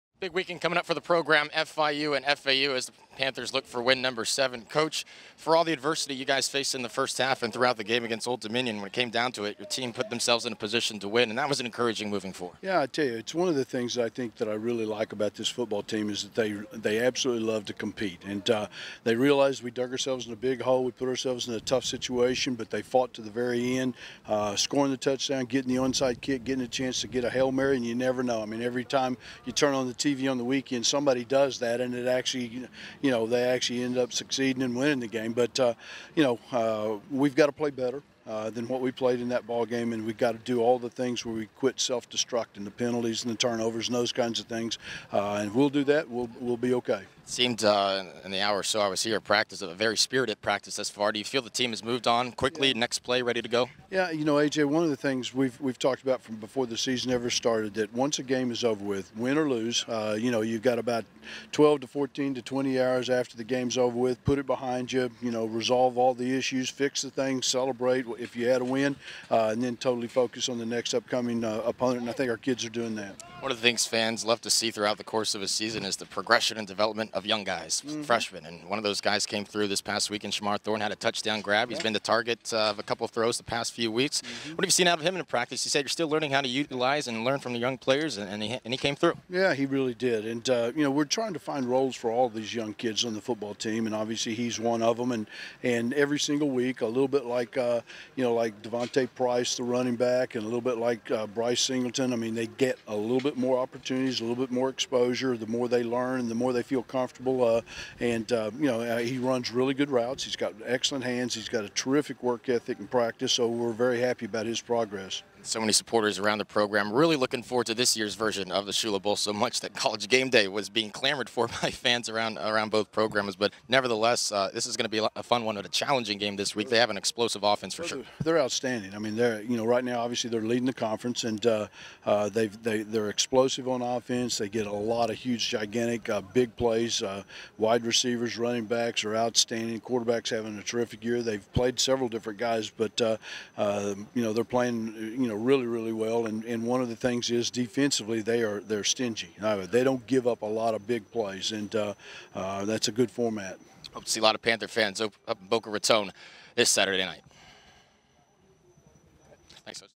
FIU Football Practice Head Coach Butch Davis Full Interview - Nov. 14, 2017
Hear the full interview from today's football practice from FIU Football Head Coach Butch Davis now!